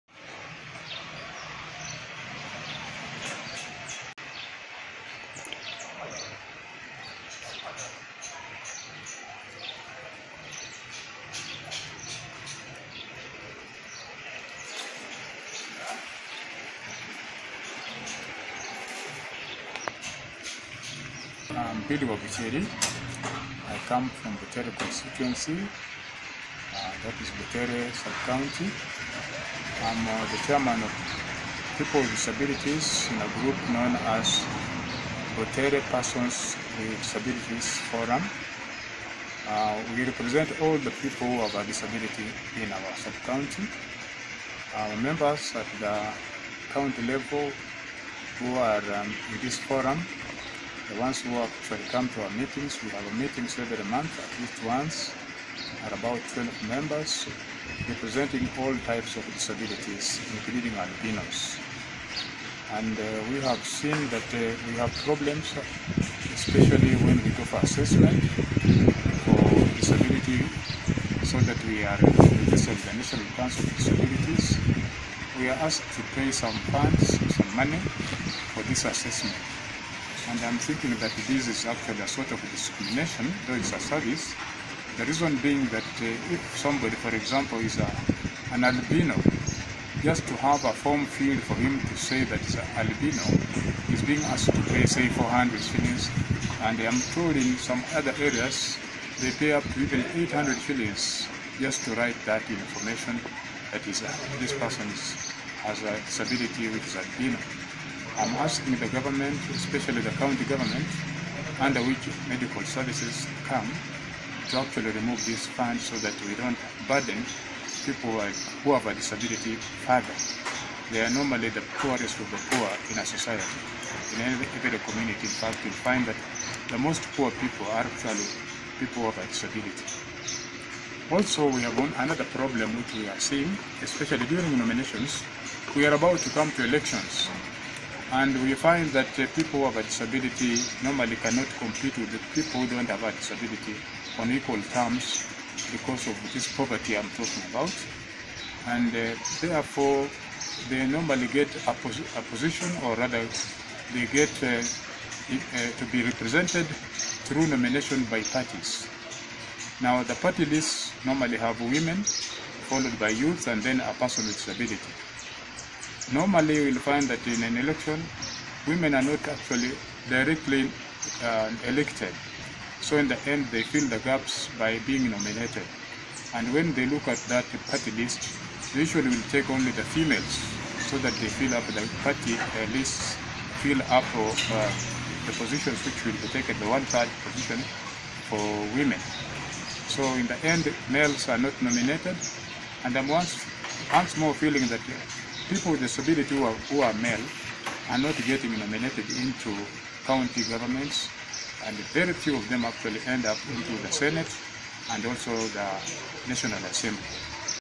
Political parties have been put on the spot by people with disabilities
Political parties have been put on the spot by people with disabilities over nomination of their representatives ahead of the next general elections.